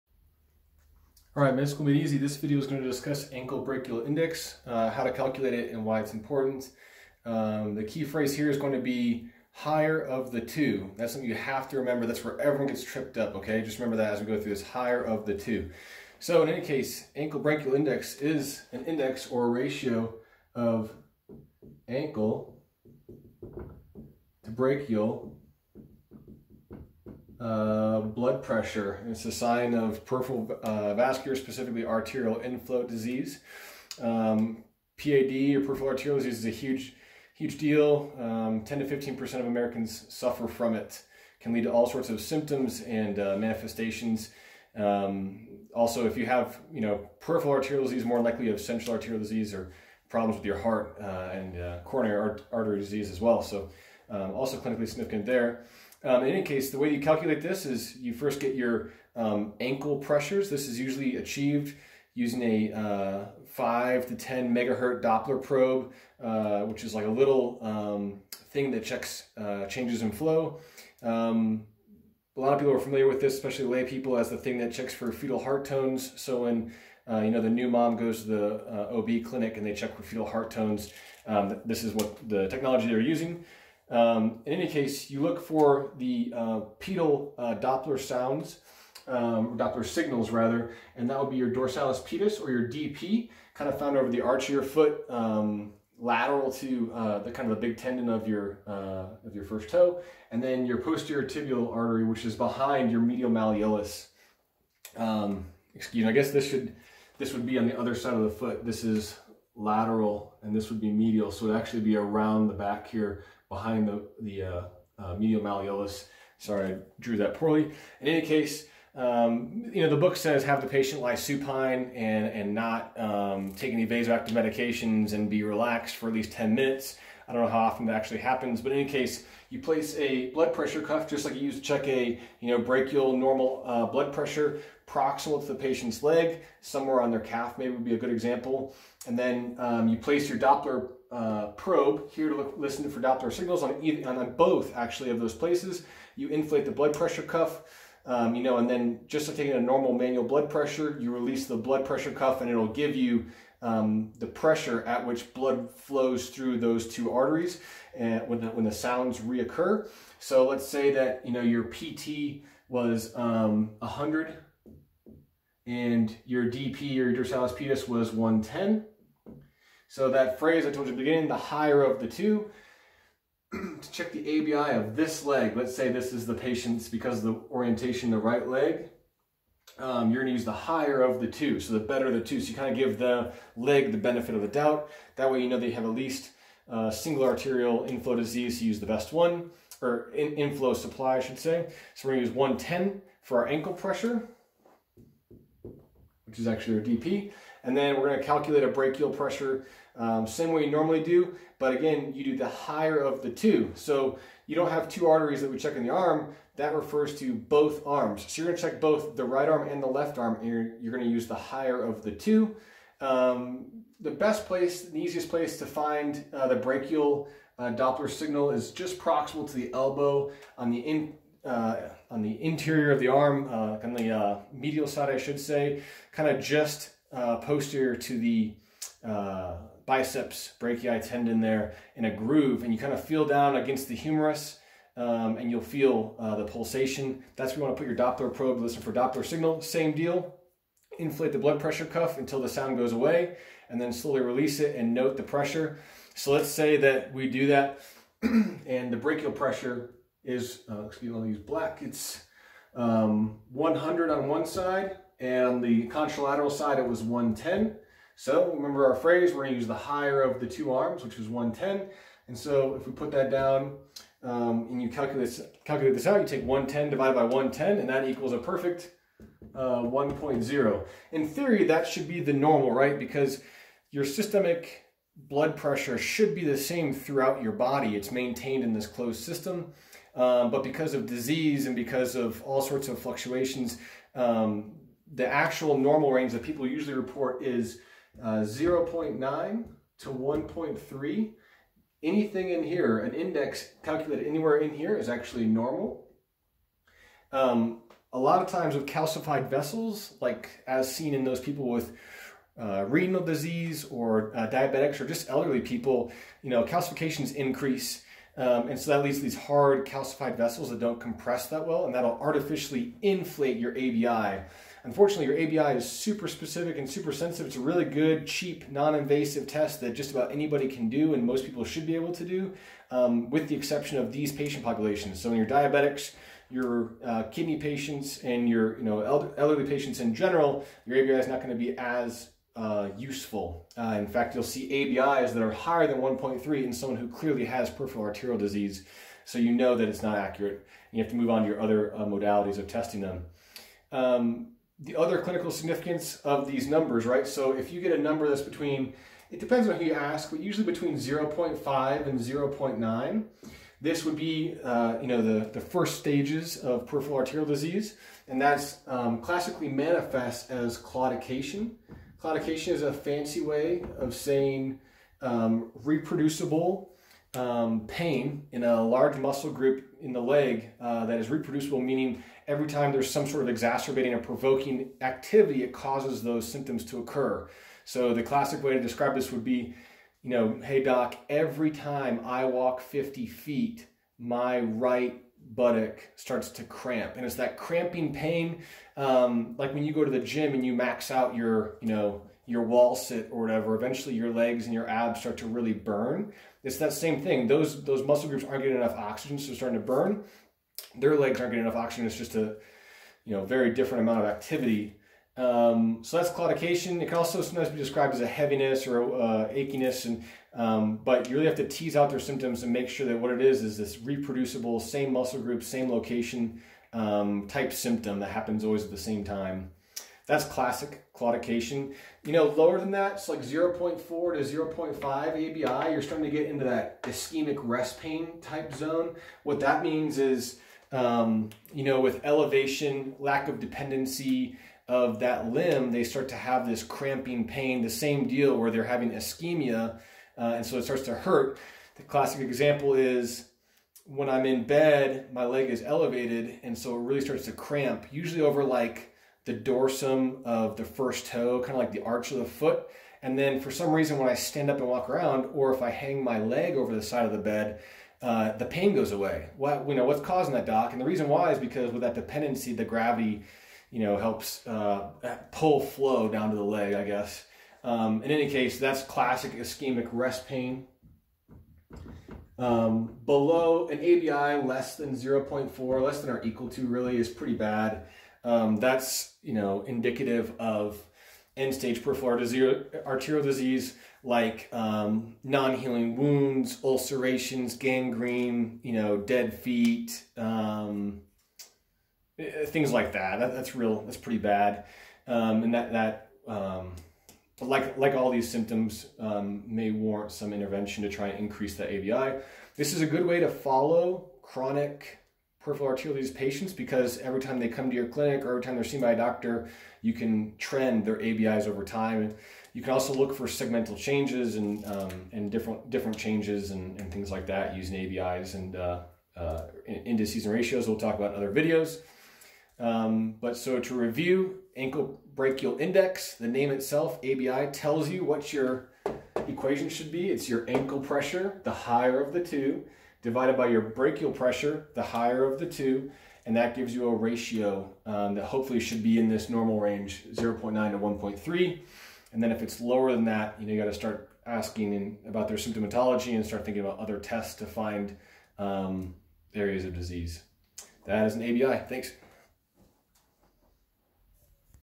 Ankle Brachial Index (ABI)- How to Measure and What it Means
Overview of the ABI and why we do it.